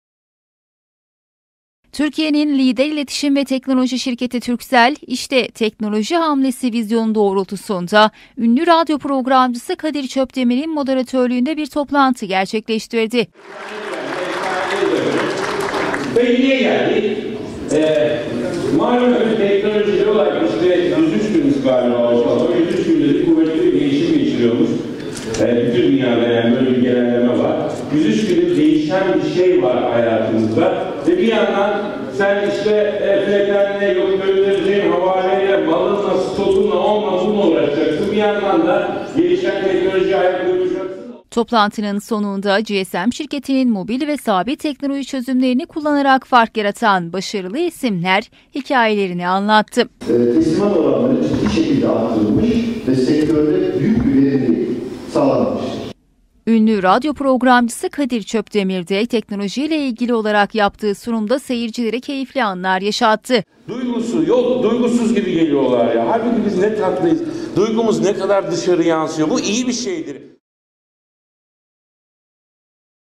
İSTİHDAMA KATKI SAĞLADIK
Anadolu'da önemli yatırımlara imza atan Türkiye’nin lider iletişim ve teknoloji şirketi, Samsun'a son 5 yılda toplam 66 milyon TL'lik yatırım yaptı. ve bugünde 'İş’te Teknoloji Hamlesi' vizyonu doğrultusunda radyo programcısı Kadir Çöpdemir’in moderatörlüğünde bir toplantı gerçekleştirildi.